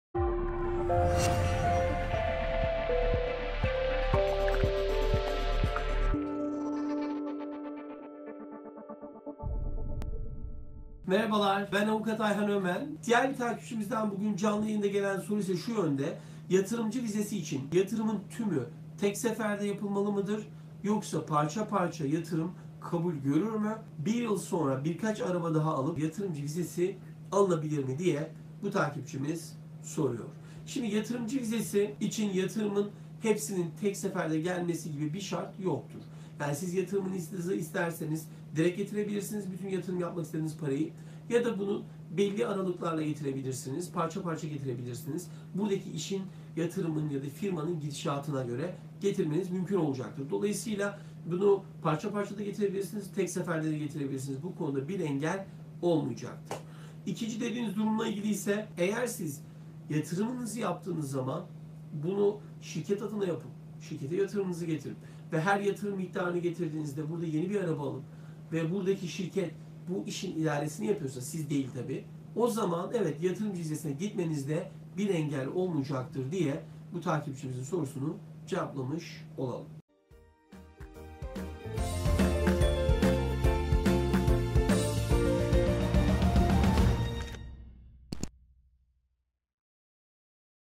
AMERİKA'DA YATIRIM YAPARKEN DİKKAT EDİLMESİ GEREKENLER NEDİR ?
Soru: Yatırımcı vizesi için yatırımın tümü tek seferde yapılmalı mıdır ? Yoksa parça parça yatırım kabul görür mü ? Bu yıl bir kaç araba alıp bir yıl sonra birkaç araba daha alıp vs. yatırımcı vizesi olabilir mi ?